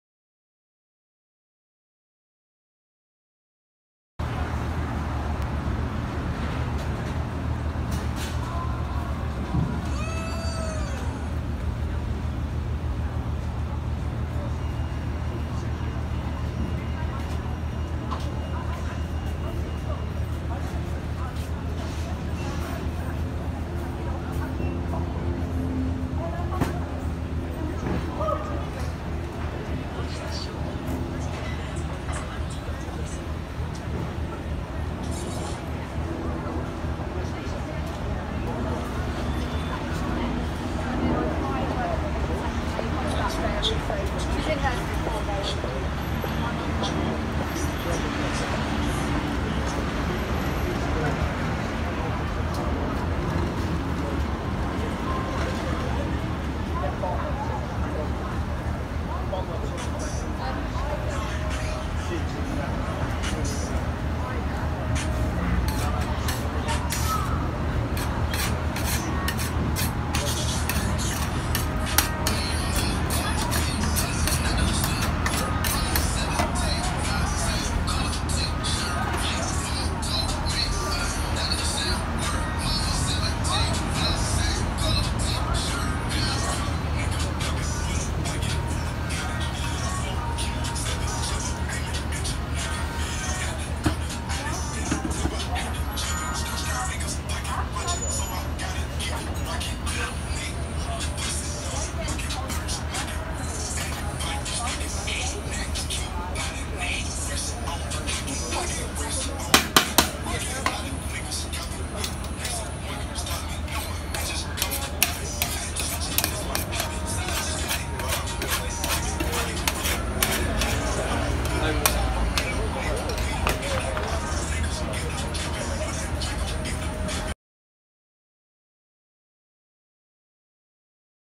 Epic Poulet Basquaise and Tartiflette. French traditional cuisine.
Amazing Tartiflette. Poulet Basquaise.Duck conflit Sandwich served with lovely with rice